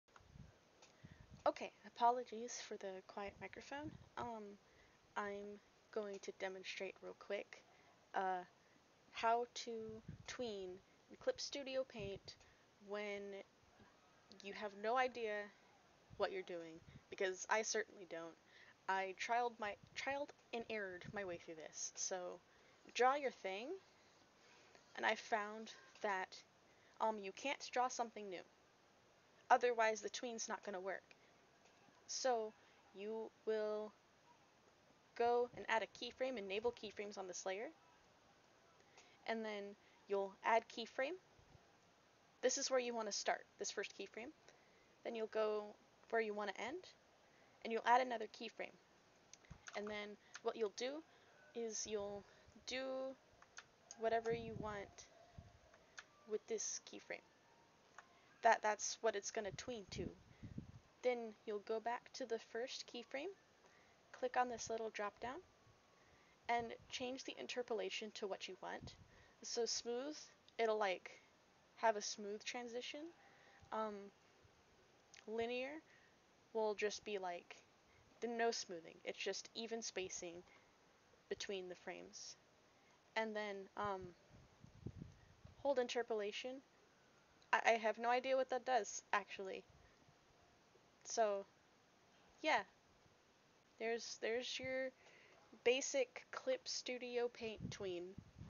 HOW TO DO A BASIC TWEEN IN CSP 1 PRO
theres apparently no good tutorial on how to make a tween in clip studio paint and all tutorials seem to assume u know how to do that already, so!!! i made this!!! as someone who also has no idea what im doing, so its basically perfect for beginners!!!